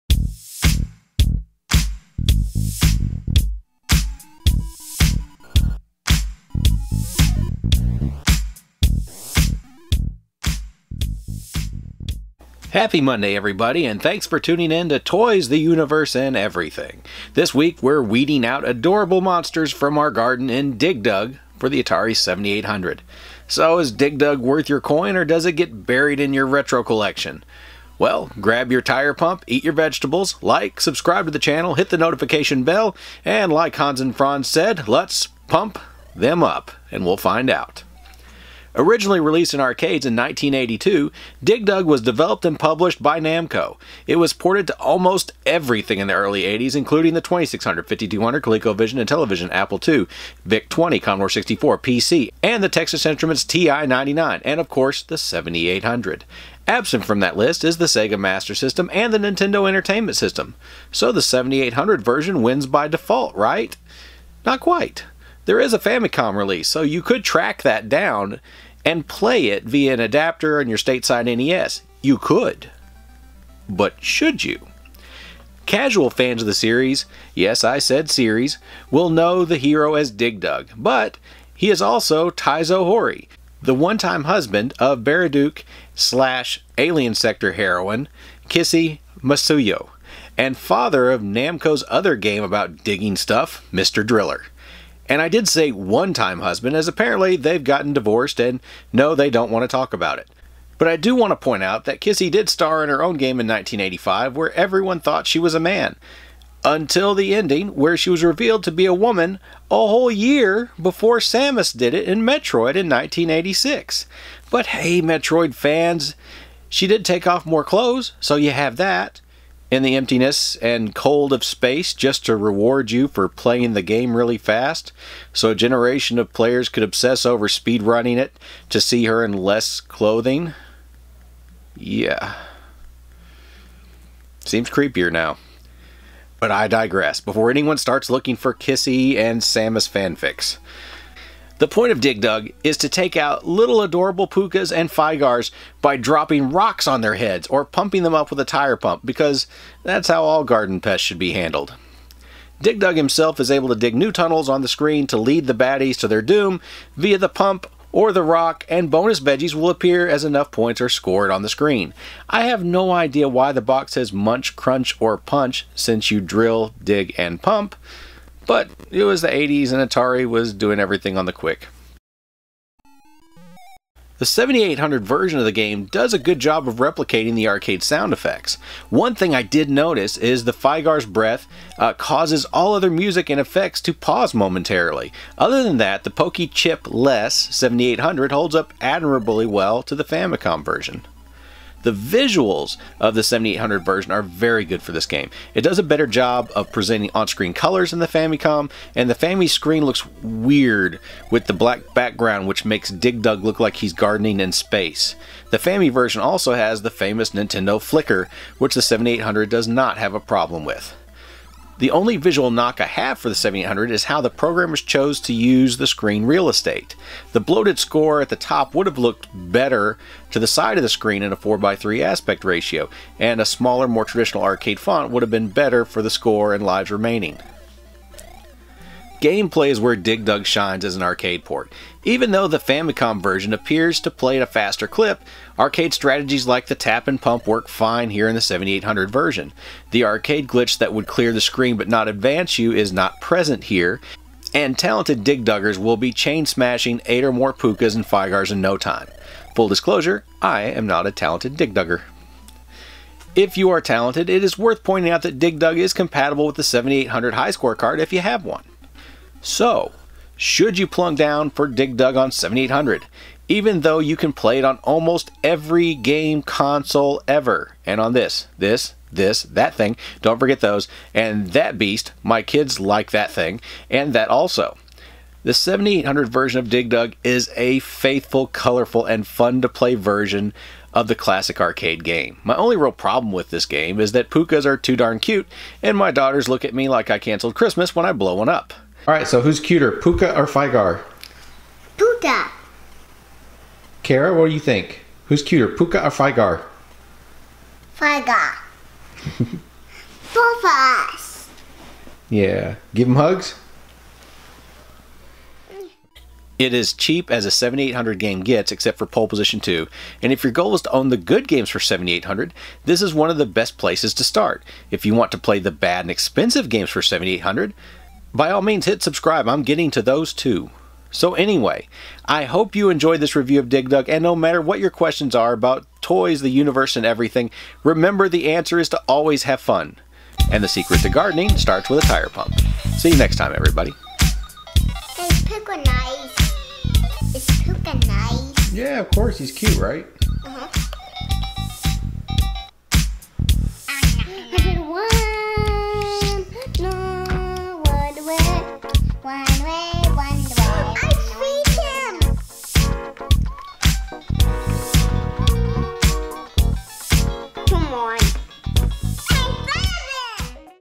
DIG DUG for Atari 7800 Review - Games, The Universe, and Everything
What is Dig Dug's real name?  Who beat Metroid's Samus to a famous video game first?  How does the Atari 7800 version rate against the Nintendo Famicom cart?  Grab your trusty tire pump and click away daring Dig Dugging viewer to find out!

What is your favorite way to play Dig Dug?